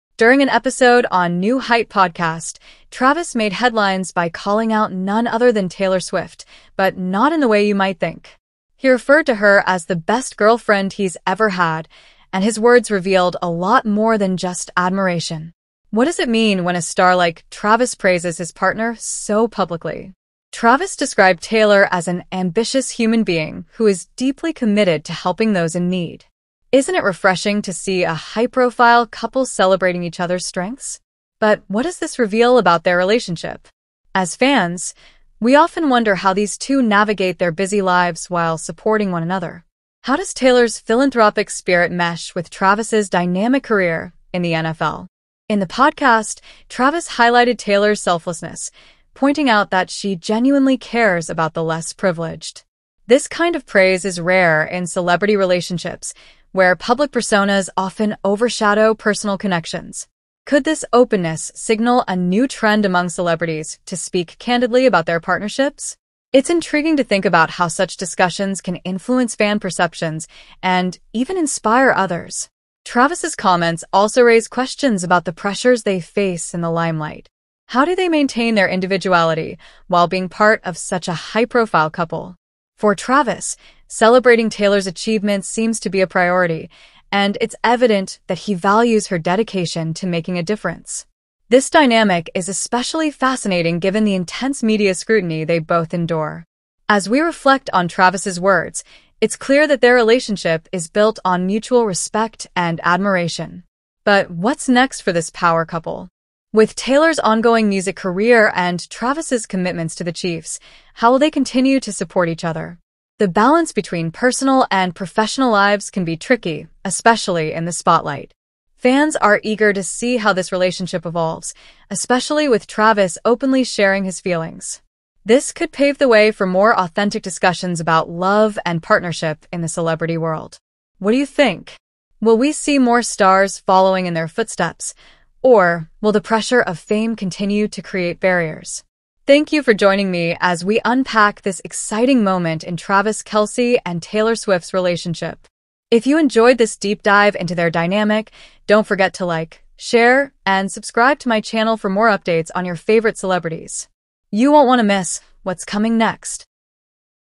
Travis Kelce Calls Out Taylor Swift
In a surprising turn of events, Travis Kelce has called out Taylor Swift, and fans are eager to know what this means for their relationship! Join us as we break down the latest comments from Travis, exploring the context and the reactions from fans and media alike. What led to this unexpected statement? Could it impact their budding romance? We’ll dive deep into the details, analyze the implications, and share what this could mean for both of their careers. Don’t miss this intriguing update!